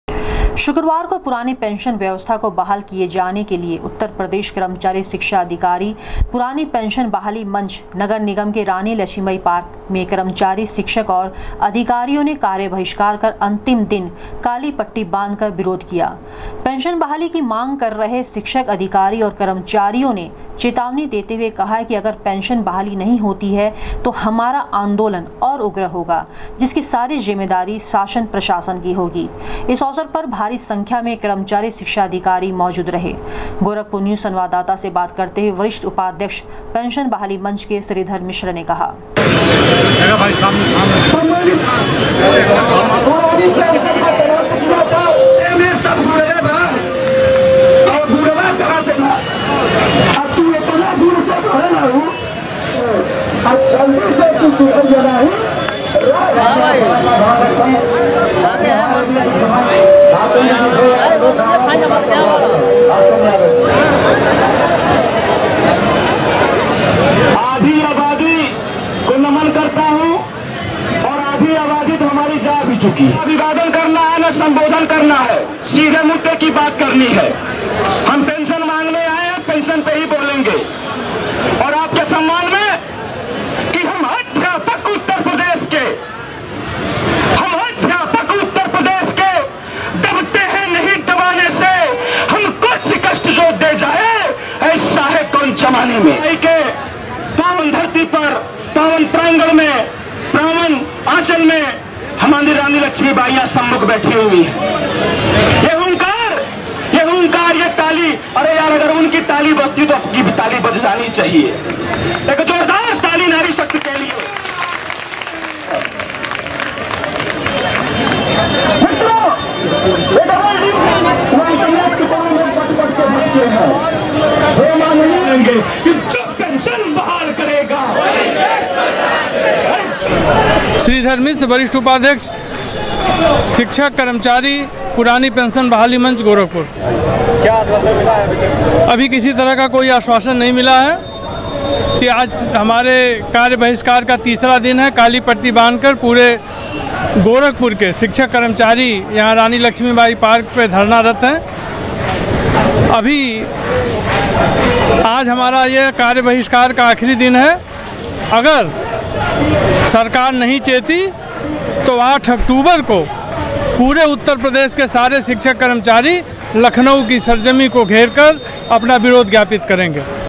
पुरानी पेंशन बहाली की मांग को लेकर उत्तर प्रदेश कर्मचारी, शिक्षक, अधिकारी पुरानी पेंशन बहाली
पुरानी पेंशन बहाली की मांग को लेकर उत्तर प्रदेश कर्मचारी, शिक्षक, अधिकारी पुरानी पेंशन बहाली मंच ने दिखाई ताकत, काली पट्टी बांधकर किया प्रदर्शन